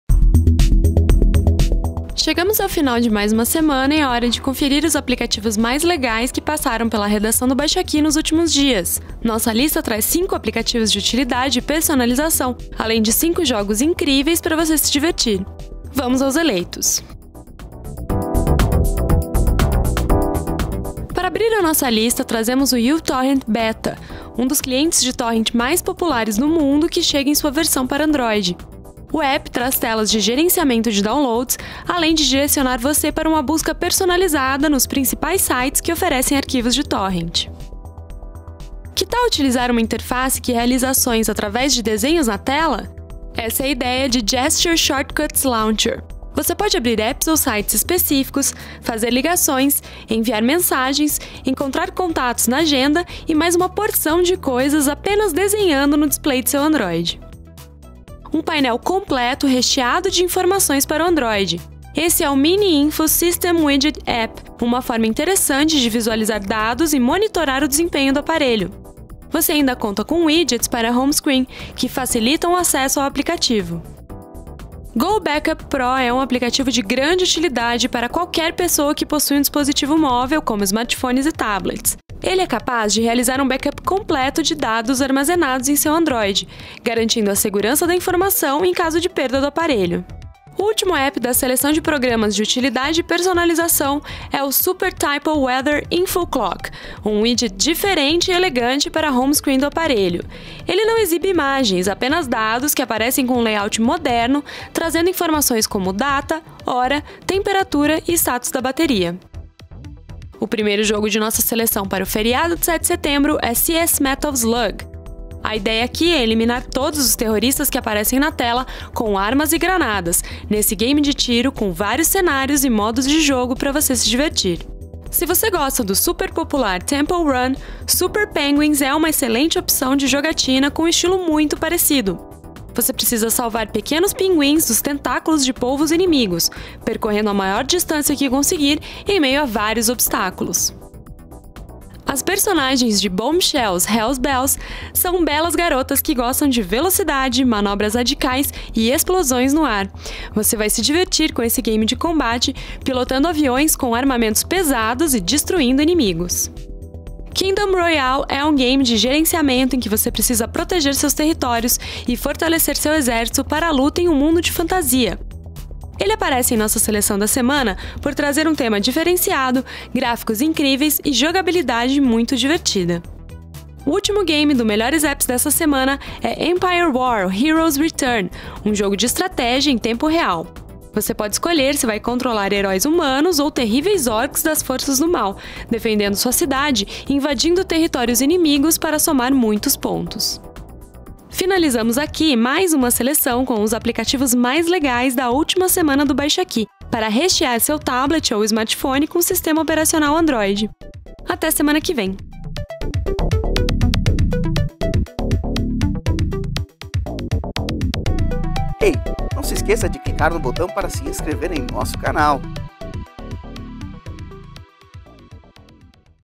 Os melhores aplicativos de Android (06/09/2012) - Baixaki
Uma seleção semanal com os aplicativos mais legais que passaram pela redação do Baixaki.

Música por Danosongs.com

::Baixaki.com.br
::Tecmundo.com.br